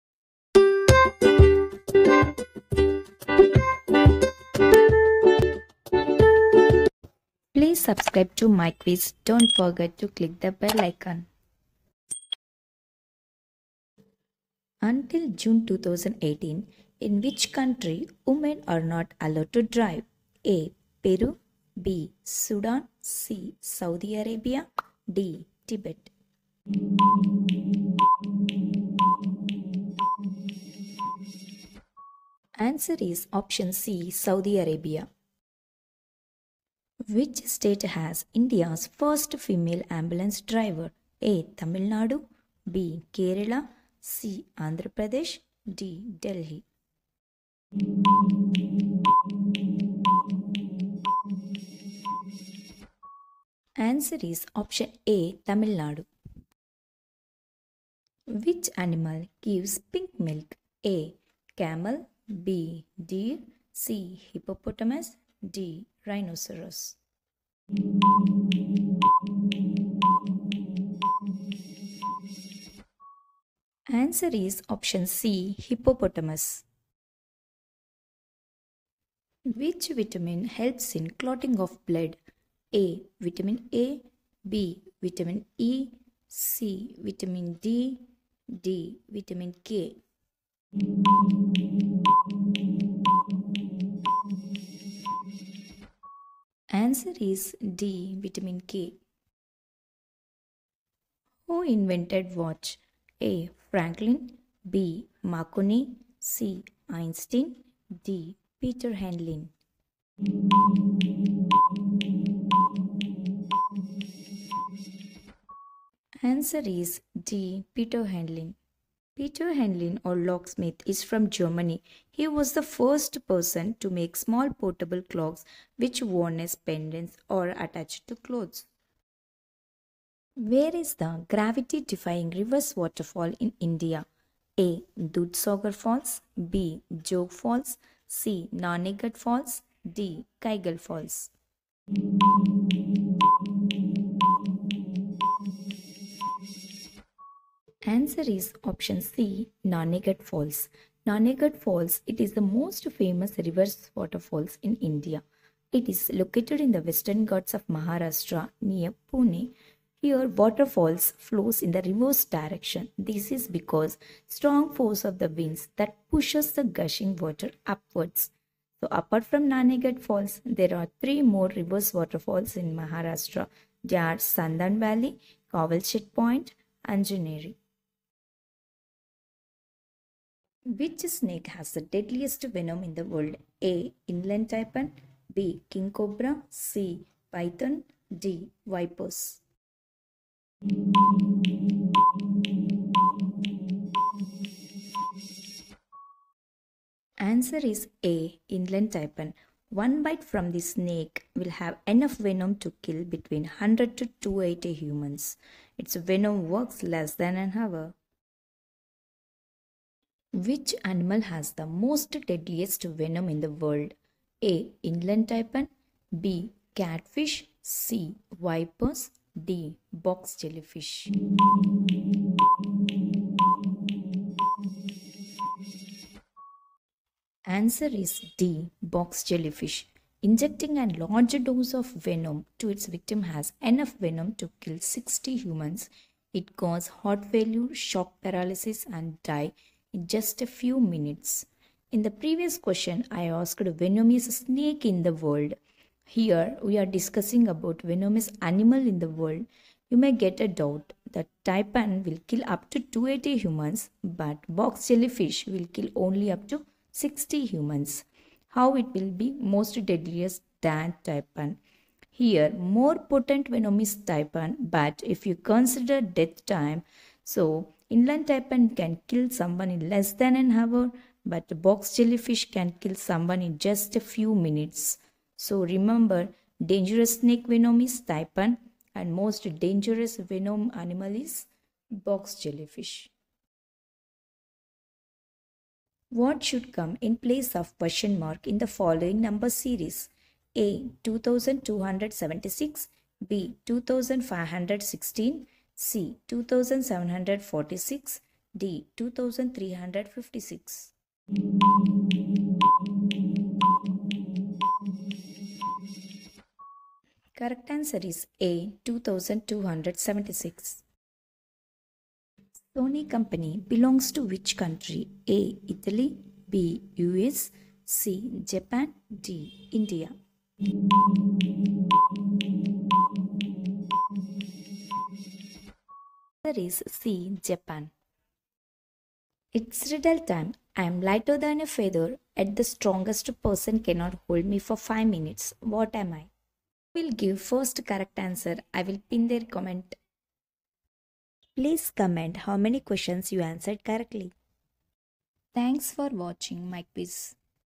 GK Questions|Where is reverse waterfall in india|Episode-7/by MI QUIZ
GK Questions|Where is reverse waterfall in india|Episode-7|MI QUIZ

I provided General Knowledge Questions with keys and Learn new things from my videos.

Queries solved:
Until June 2018,which country women are not allowed to drive

Which state has India's first female ambulance driver

Which animal gives pink milk

Which vitamin helps in the clotting of blood

Where is the gravity defying reverse waterfall in India

Which  snake has the deadliest venom in the world

Which animal has the most deadliest venom in the world
What should come in place of question mark in the following number series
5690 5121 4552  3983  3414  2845

Sony company belongs to which country